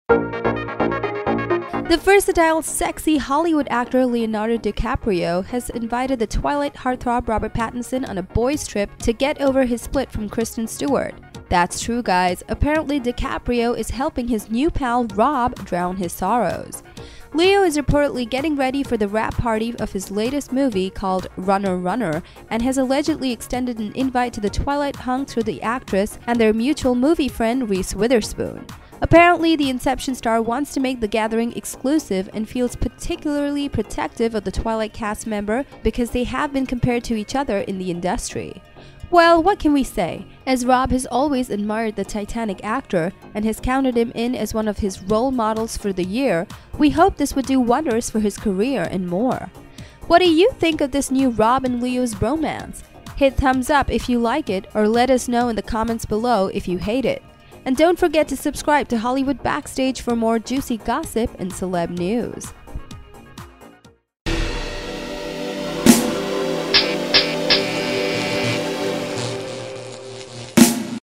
Robert Pattinson and Leonardo DiCaprio's new found BROMANCE !
The versatile, sexy, Hollywood actor Leonardo DiCaprio has invited the twilight heartthrob, Robert Pattinson on a boys' trip to get over his split from Kristen Stewart. That's true guys; apparently DiCaprio is helping his new pal Rob drown his sorrows!